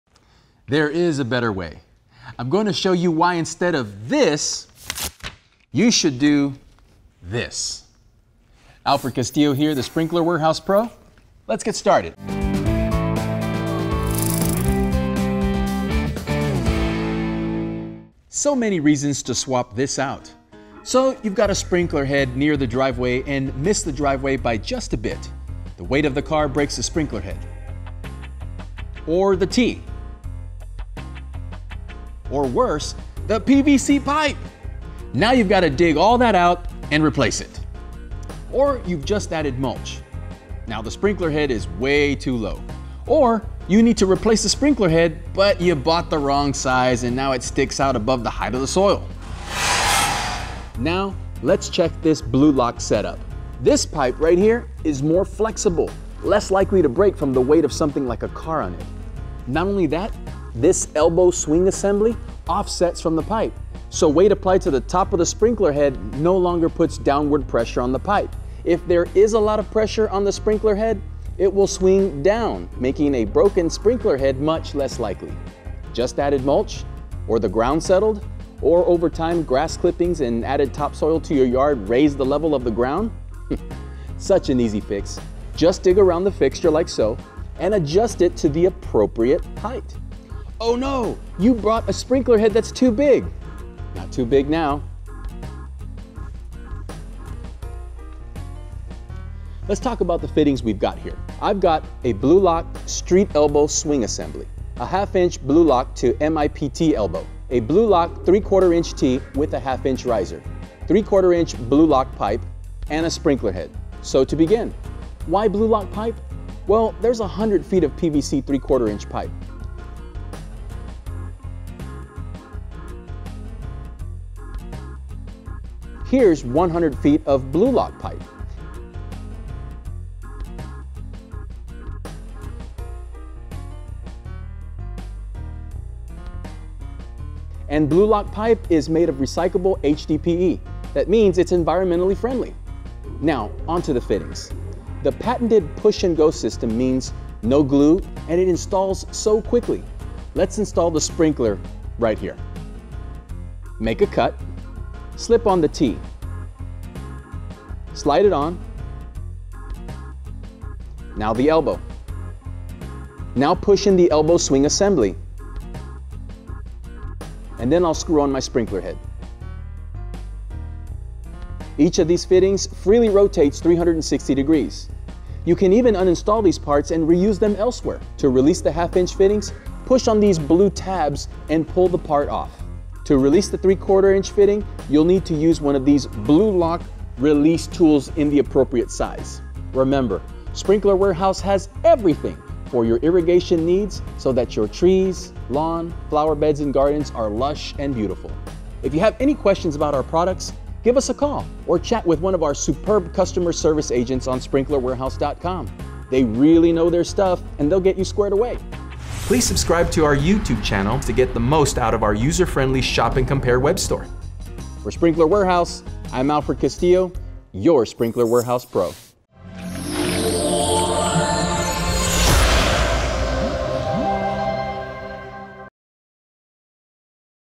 Installing A Sprinkler Head With Blu Lock
Alfred explains why you should use Blu-Lock pipe and fittings when installing your sprinkler system and its advantages over standard PVC piping and fittings.